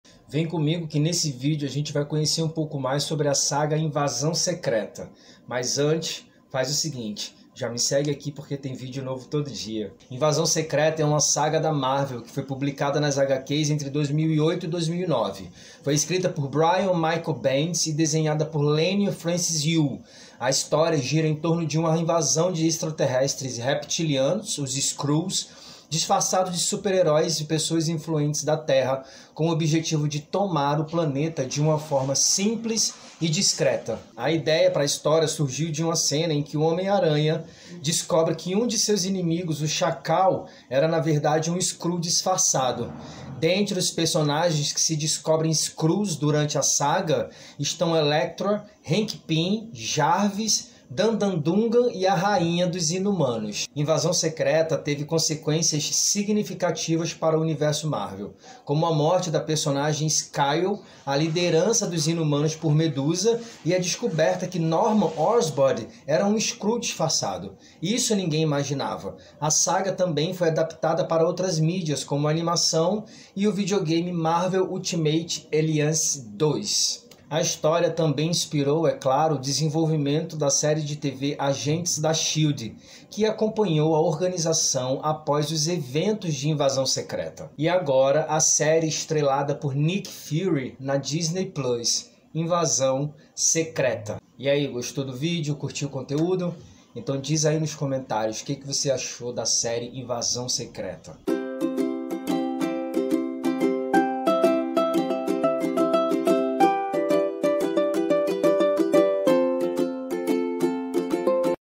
Invasão Secreta 🦸 ( História Completa Hqs / Série )
Em Invasão Secreta, o personagem Nick Fury, ex-diretor da SHIELD, volta à Terra depois de descobrir os planos ligados a uma raça alienígena, os Skrulls, empenhados em dominar o planeta. Para isso, ele precisa reunir aliados de longa data.há 2 dias